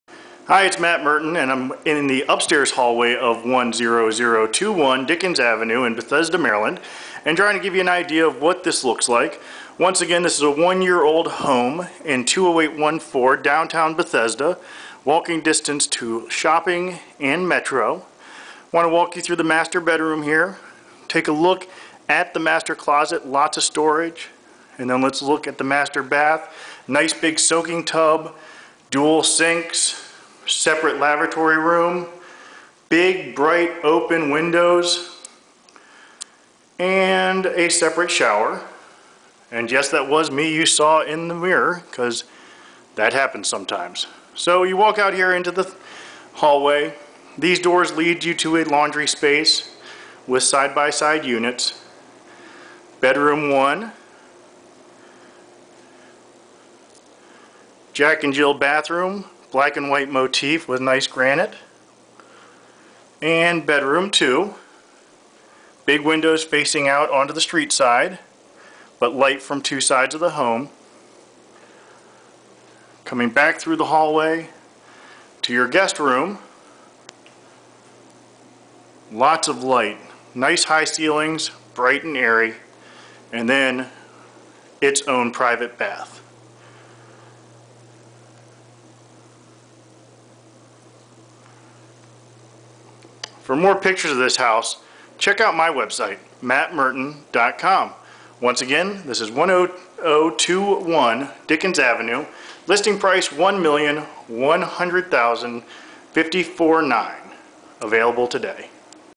10021 Dickens Ave Just Listed Hosted by Matt Murton(part 2)
Just listed, 10021 Dickens Ave, Bethesda, MD 20814. 5 bedrooms, 4.5 Baths, over 4500 sqft, one car detached garage...listed for $1,154,900 more pictures